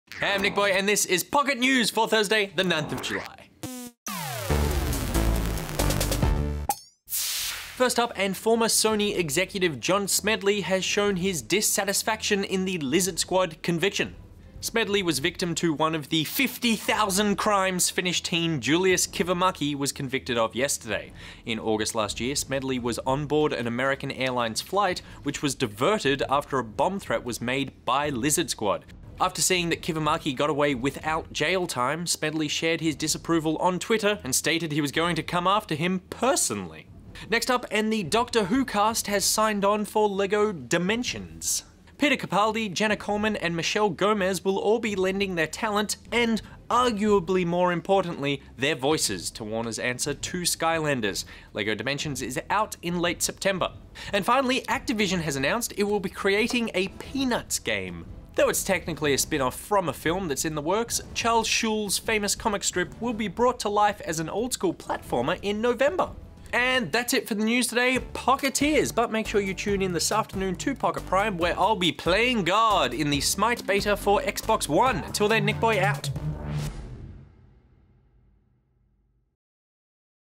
Unhappy John Smedley, Doctor Who Dimensions - GG Pocket - 09/07/15
John Smedley will pursue further action against Julius Kivimaki, Doctor Who characters in LEGO Dimensions and Snoopy coming to consoles!